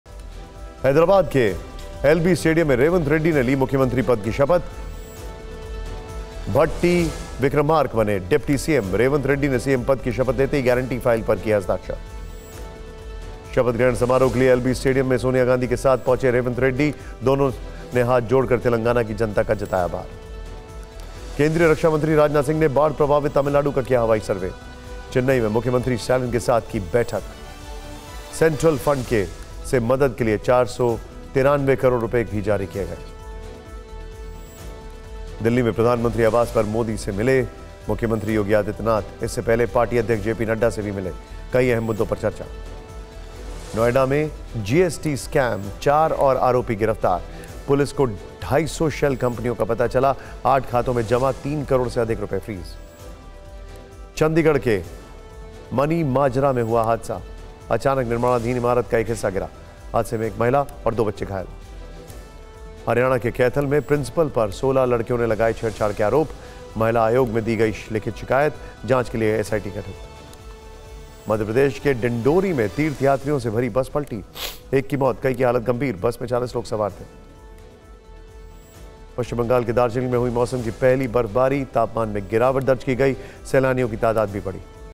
Hyderabad के एलबी स्टेडियम में Revanth Reddy ने ली मुख्यमंत्री पद की शपथ
Hyderabad के एलबी स्टेडियम में Revanth Reddy ने ली मुख्यमंत्री पद की शपथ...भट्टी विक्रमार्क बने डिप्टी सीएम...रेवंत रेड्डी ने सीएम पद की शपथ लेते ही गारंटी फाइल पर किए हस्ताक्षर...